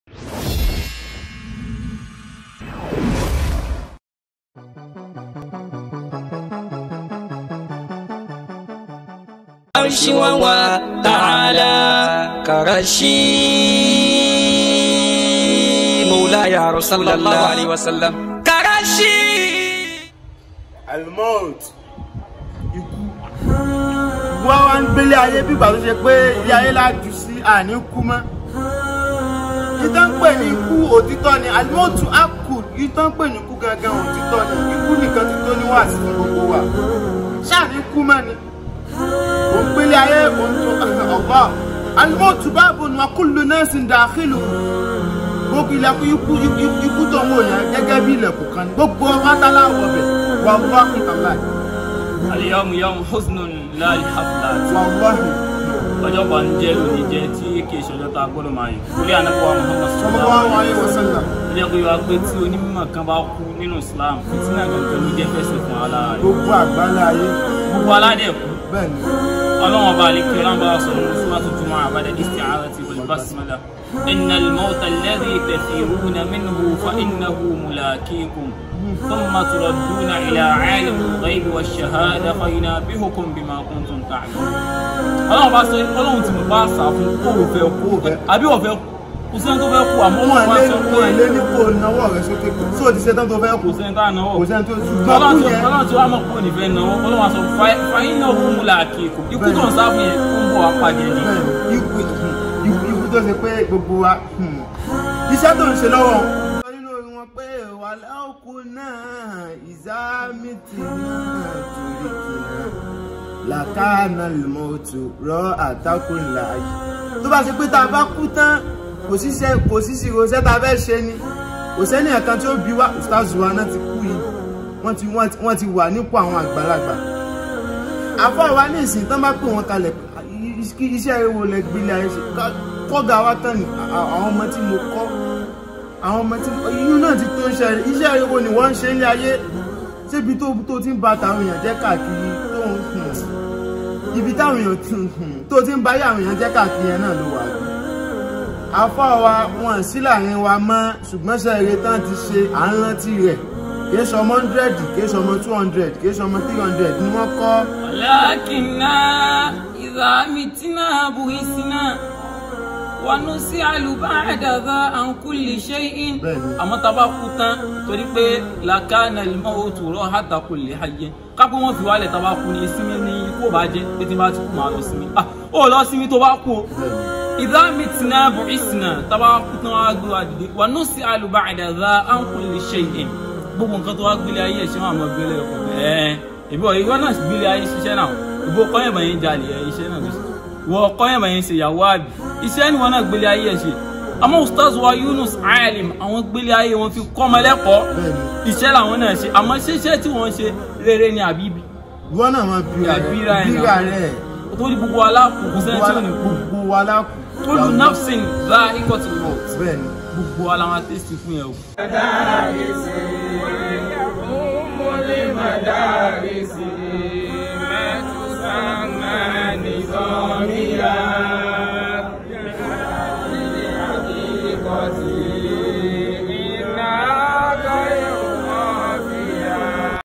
Right Minded: Episode 6
DEATH IS INEVITABLE: (Tribute to Ustaz Yunus Aleem) 😢🥺

LECTURE 🗣 Ismaheel Habeeblahi & AbdKareem Nuraen

Visual 🎥.  On Standby

Jumuah Mubarak from المحاضرة الإسلامية